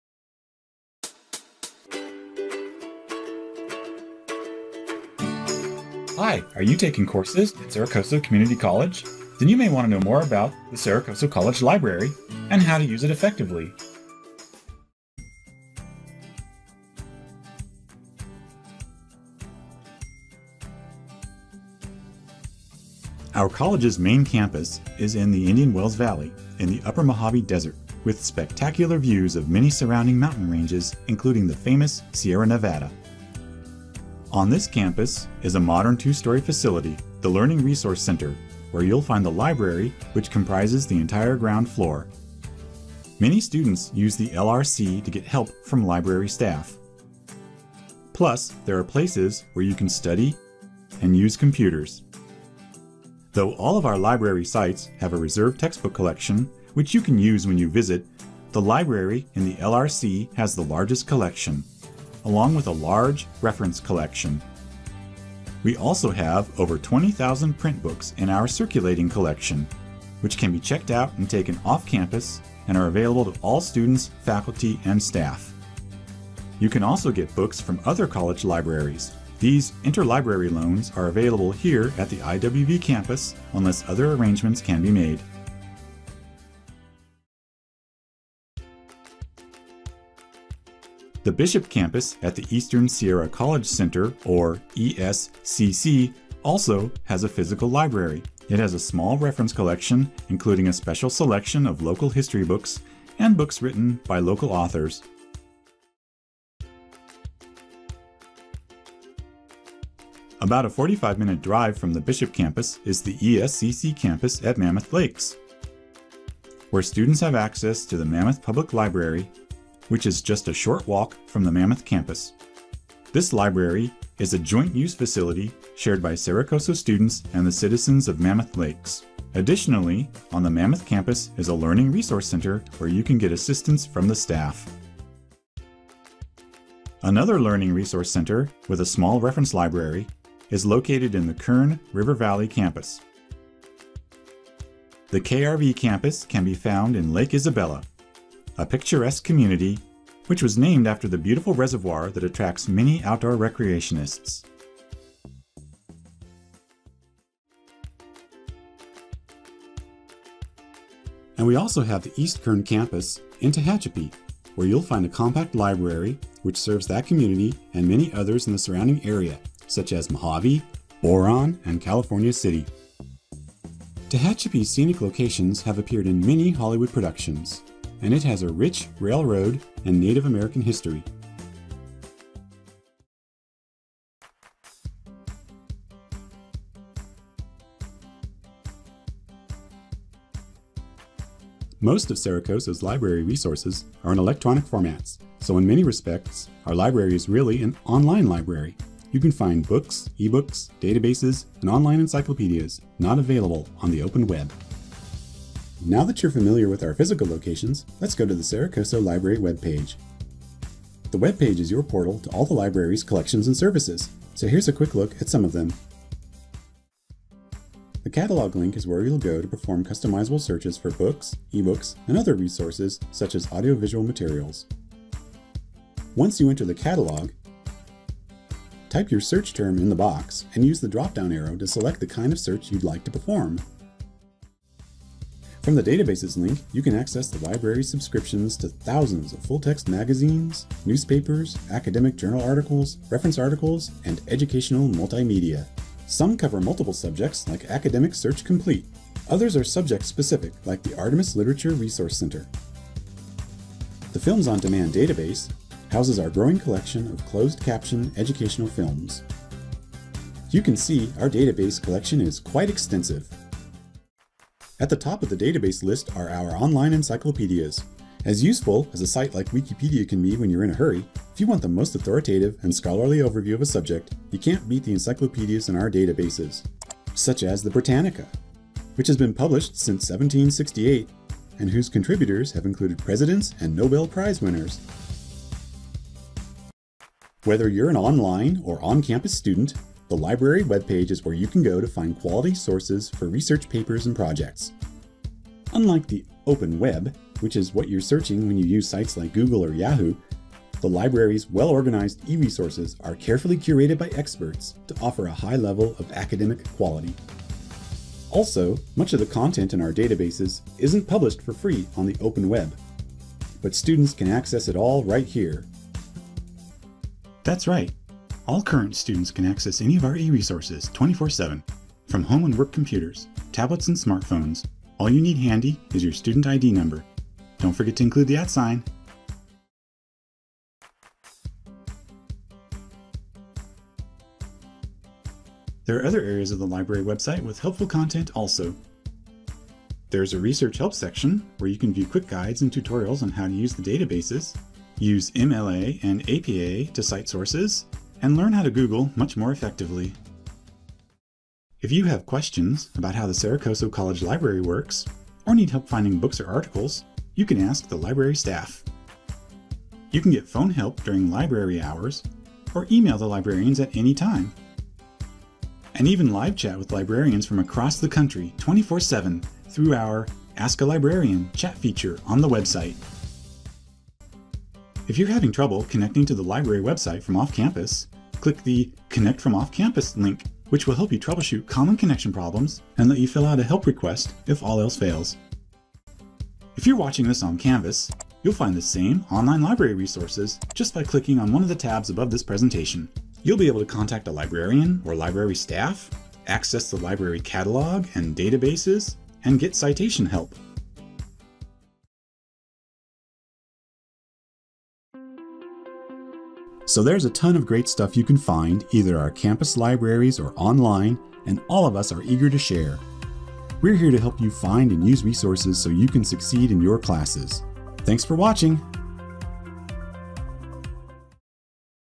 Welcome to the Library
An introduction to the Cerro Coso Library and its resources.

Created in 2018 by Davis Kunz for the Cerro Coso Community College Library.